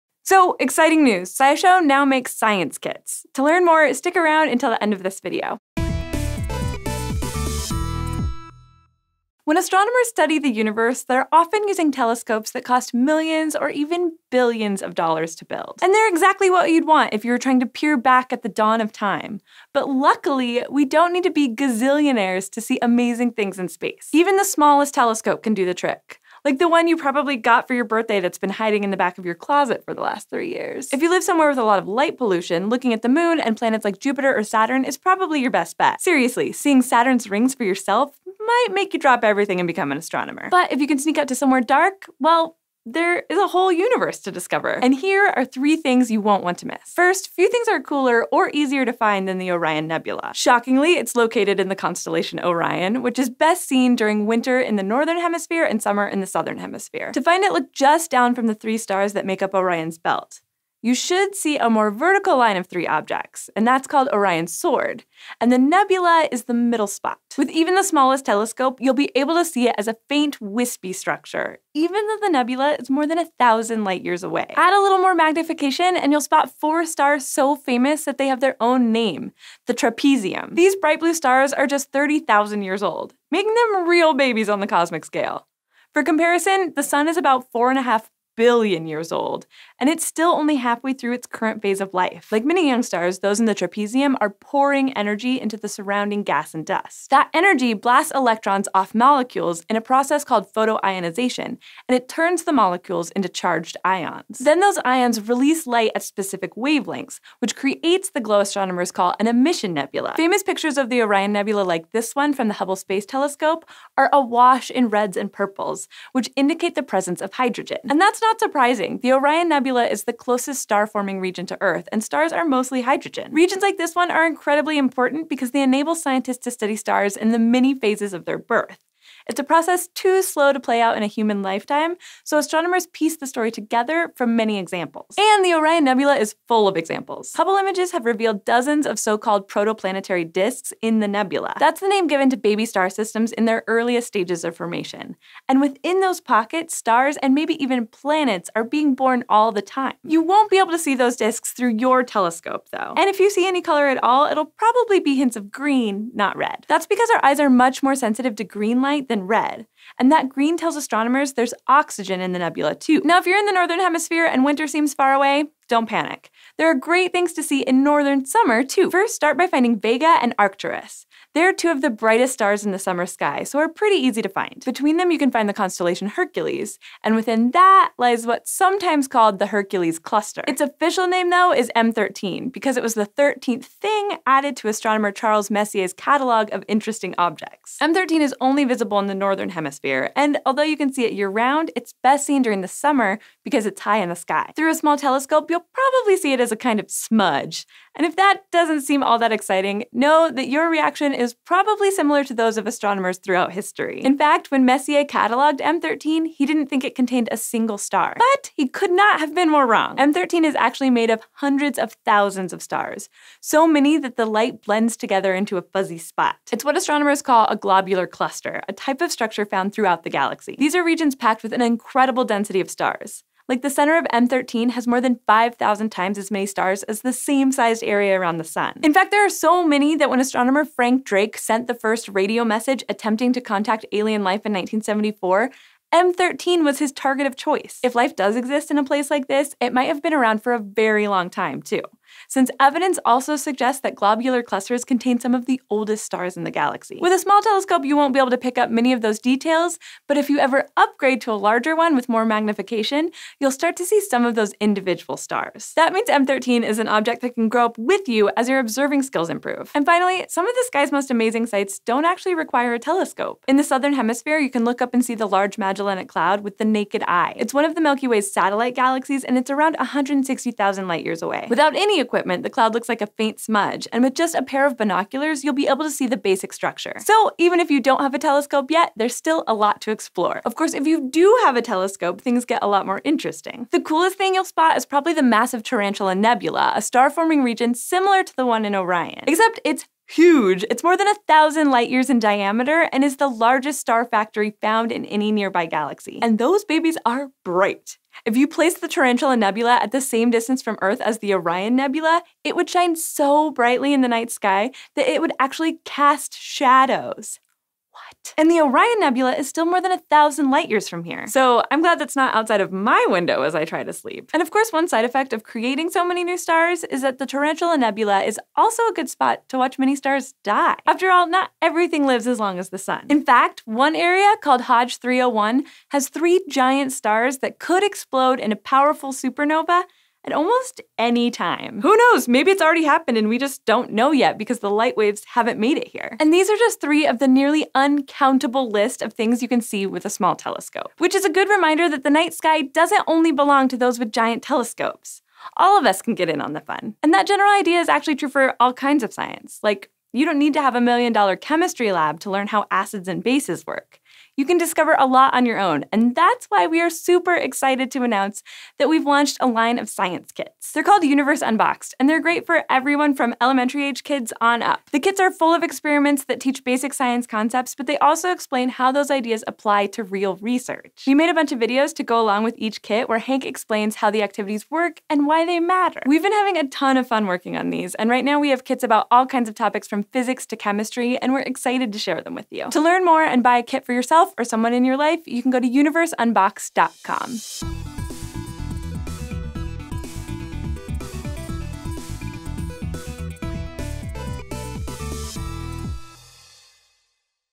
3 Amazing Objects to Check Out with Your New Telescope
When astronomers study the universe, they’re often using telescopes that cost millions or even billions of dollars to build. Luckily for the rest of us, there are still plenty of incredible things to see in the sky with the more affordable models.

We make science kits now! to learn more, order one online, or find them in a store near you.

Host: Caitlin Hofmeister